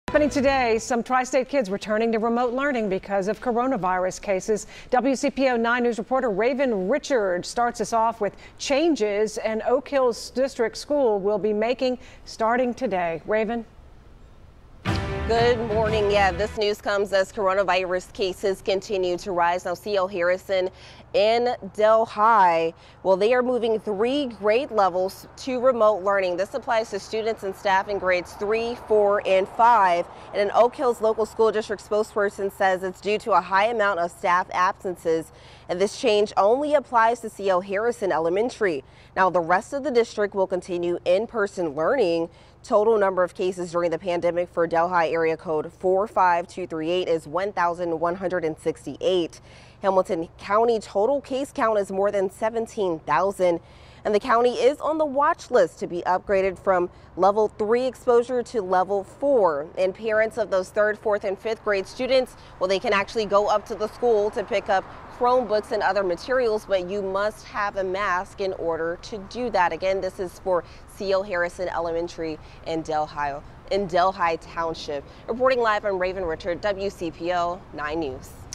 Three grades in Oak Hills elementary school to quarantine after high staff absences
Students in three grades at C.O. Harrison Elementary School in Delhi Township will quarantine and move to remote learning for two weeks after a high level of staff absences, according to a spokesperson for Oak Hills Local School District.